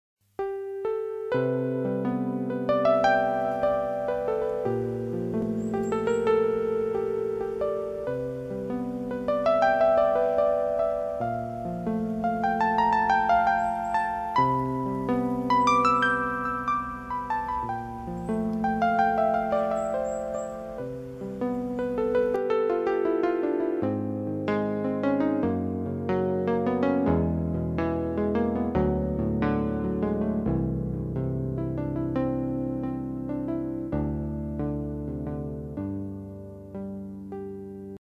Becherlinge im Wald - Otidea onotica
Many mushrooms in North German forest.  1. Oktober 2015.